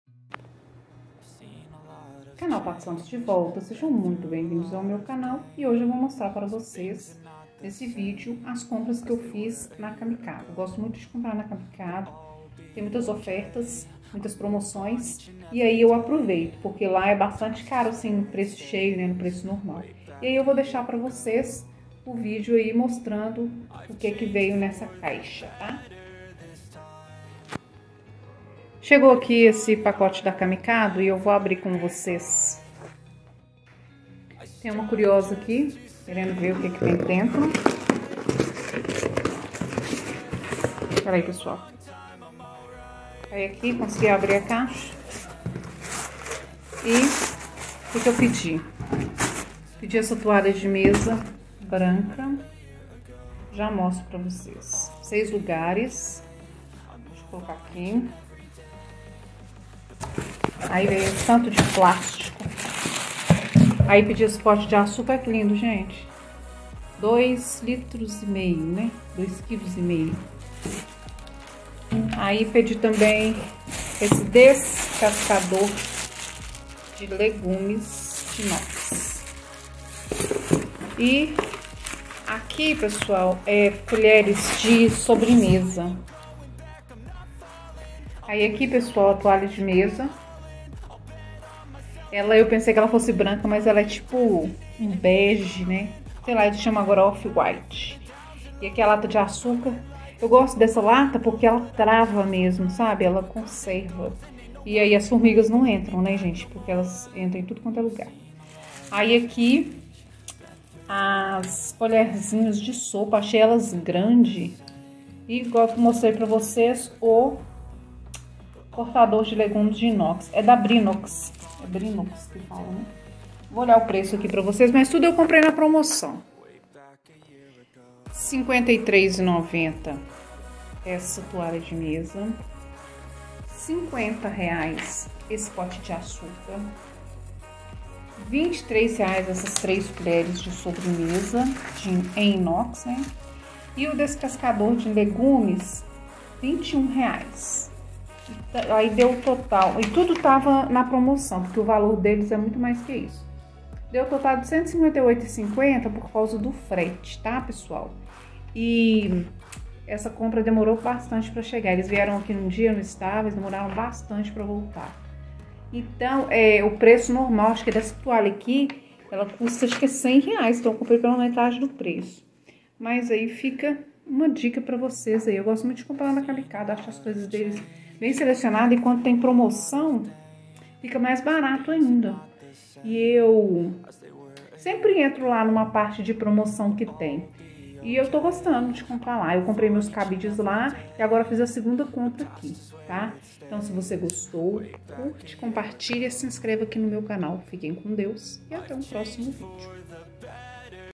COMPRAS NA CAMICADO
Venha conferir nesse vídeo compras para o lar feitas na Camicado uma loja de utilidades domésticas e muito mais. Itens de qualidade e bom gosto.